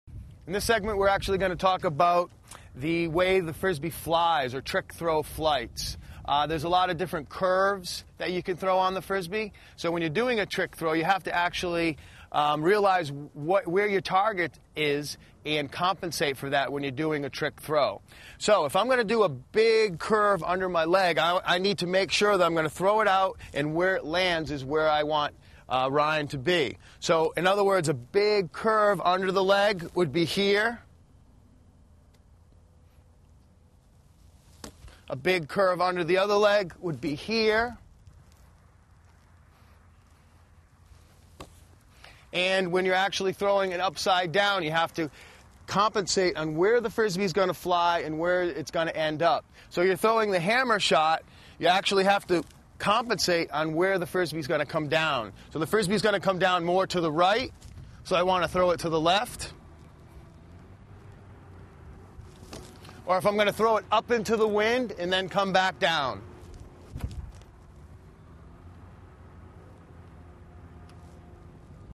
Freestyle Frisbee: Flight Patterns
Freestyle Frisbee: Flight Patterns. Part of the series: Freestyle Frisbee Throws & Tricks. The flight pattern of your disc, in freestyle Frisbee, is important for accurately hitting your target and throwing more precise Frisbee trick throws. Learn more about flight patterns and freestyle Frisbee from a freestyle Frisbee champion in this free Frisbee video.